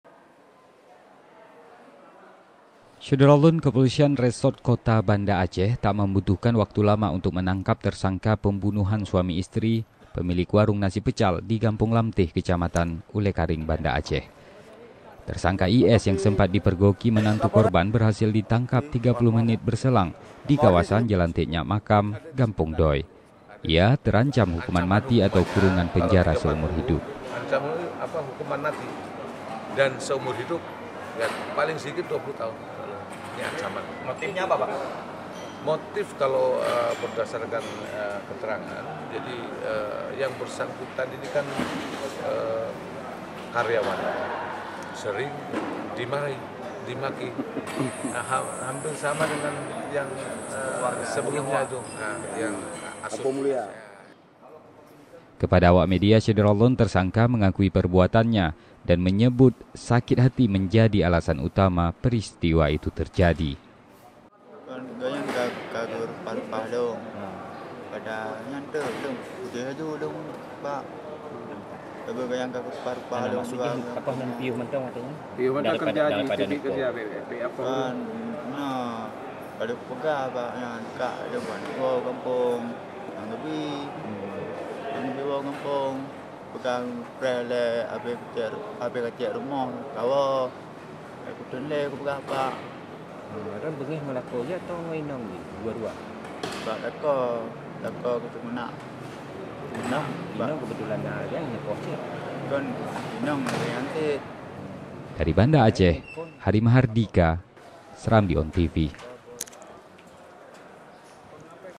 Terancam Hukuman Mati, Begini Pengakuan Tersangka Kasus Pembunuhan Suami-Istri Pedagang Nasi Pecal
SERAMBITV.COM, BANDA ACEH - Kepolisian Resort Kota Banda Aceh tak membutuhkan waktu lama untuk menangkap tersangka pembunuhan suami-istri pemilik warung nasi pecal, di Gampong Lamteh, Kecamatan Ulee Kareng, Banda Aceh.

Tersangka IS yang sempat dipergoki menantu korban berhasil ditangkap 30 menit berselang, di kawasan Jalan T Nyak Makam, Gampong Doy.

Ia terancam hukuman mati atau kurungan penjara seumur hidup.

Sementara kepada awak media, tersangka mengakui perbuatannya, dan menyebut sakit hati menjadi alasan utama peristiwa itu terjadi.